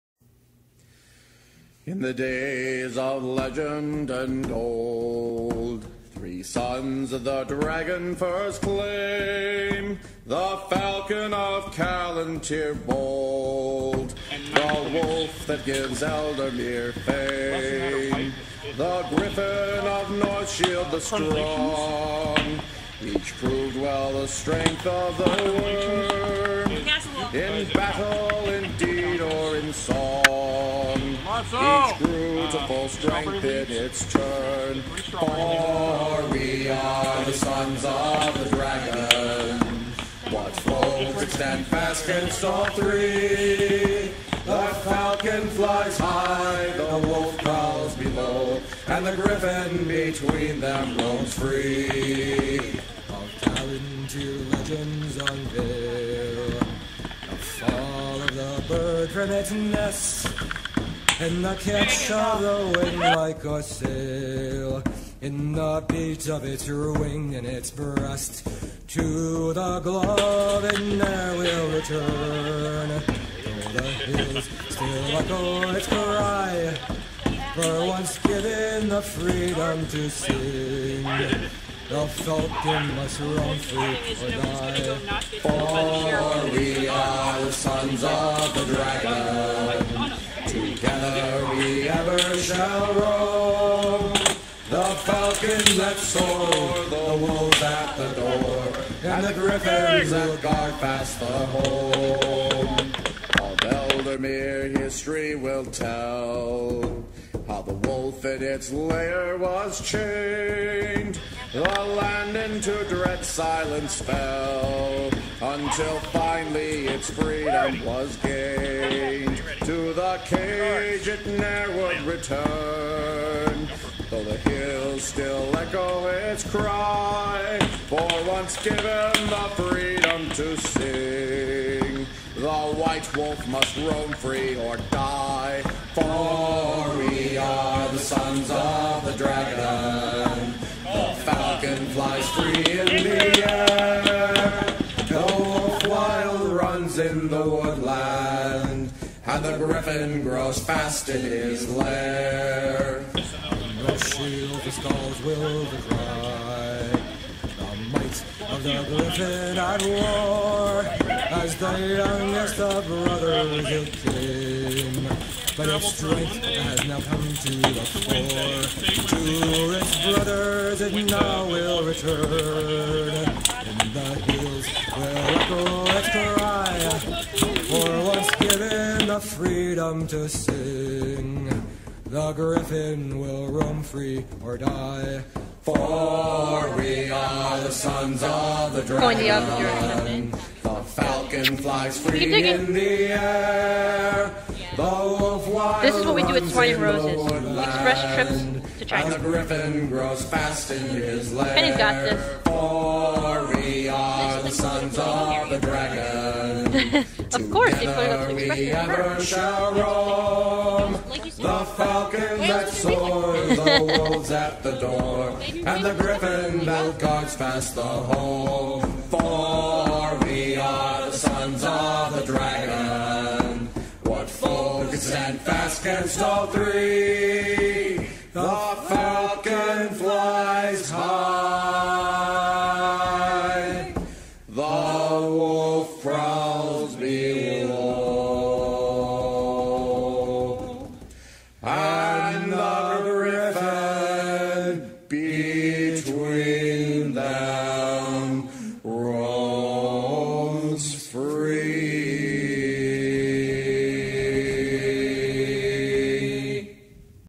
Swine and Roses 2014
A Set of fights during the Salamander Tournament.